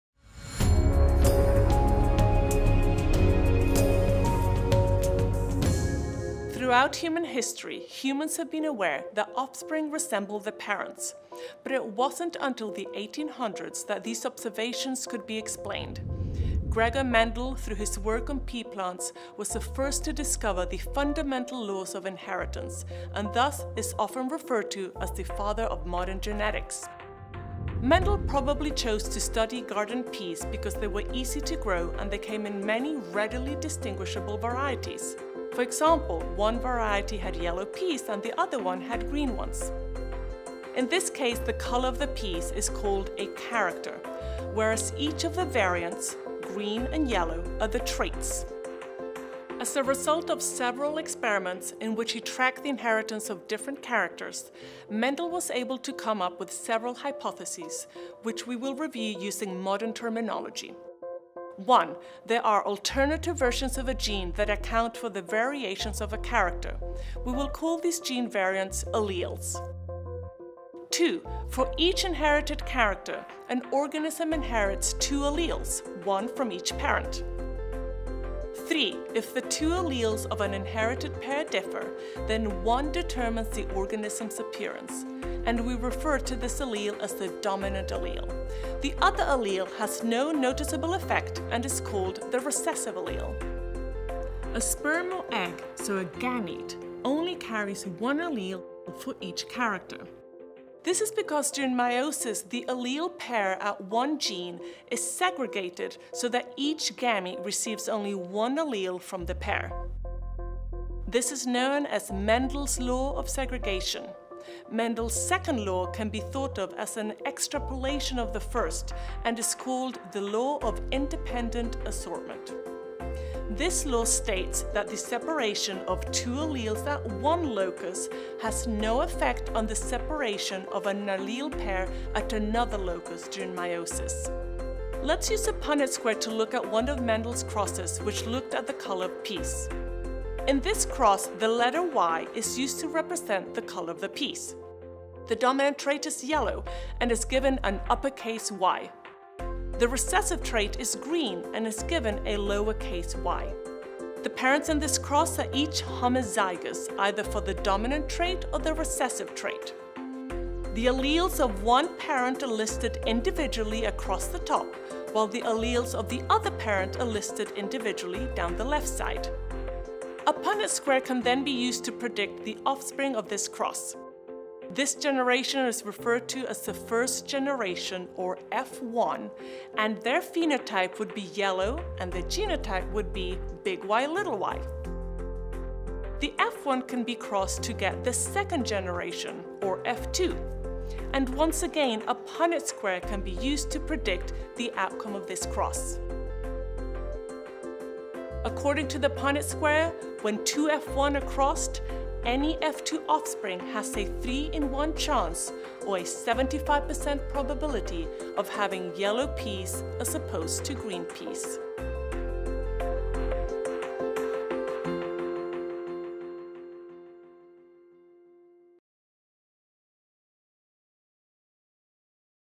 Genotypes, Phenotypes, and Alleles | Biology
Summarize videos instantly with our Course Assistant plugin, and enjoy AI-generated quizzes Learn all about genotypes, phenotypes, and alleles in just a few minutes! Jessica Pamment, professional lecturer at DePaul University, walks through the relationships between alleles (versions of genes), genotypes (genetic makeup of the organism), and phenotypes (observable characteristics of an organism).